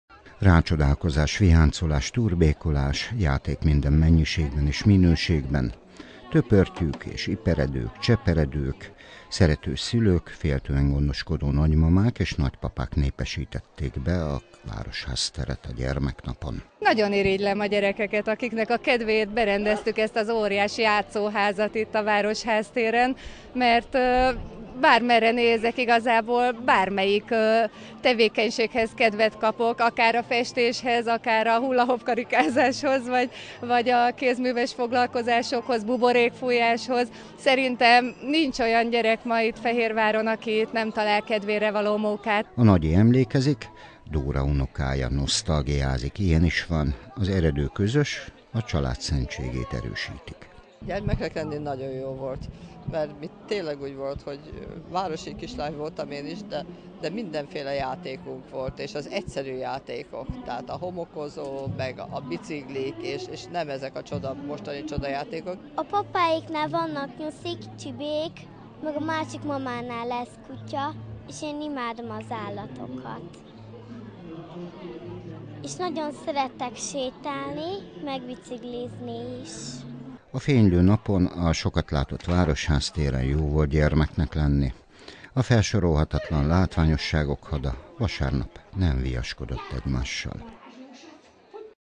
Gyereknap a belvárosban
Május utolsó vasárnapján a Városház teret vették birtokba a fehérvári gyermekek és szüleik. A hagyományoknak megfelelően látványos, színes programokkal várták a szervezők a családokat a gyermeknap alkalmából.